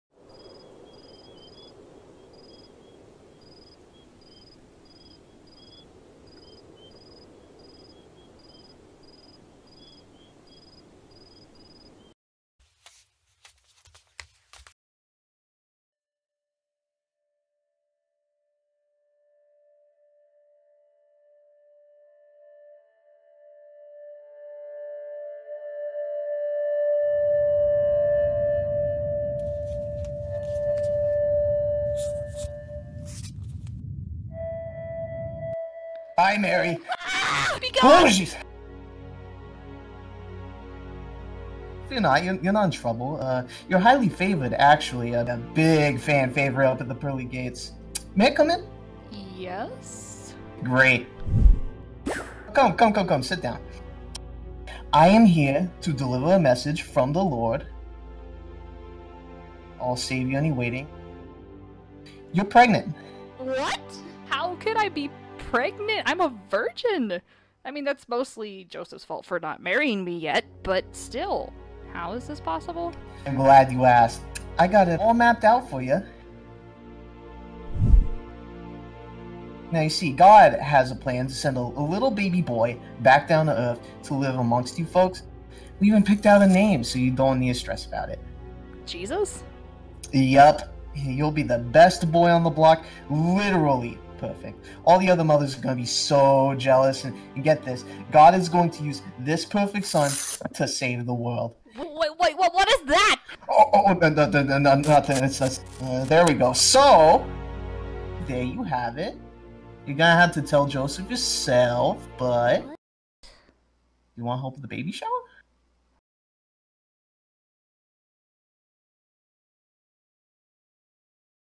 Films📽️ The Announcement - CSUF student animatic
A parody retelling of The Annunciation in the Bible
Spring 2020 animatic from Animation Preproduction at CSUF

Hardware: Huion Kamvas GT 221 Pro
Software: Clip Studio Paint EX
Editing: Filmora Pro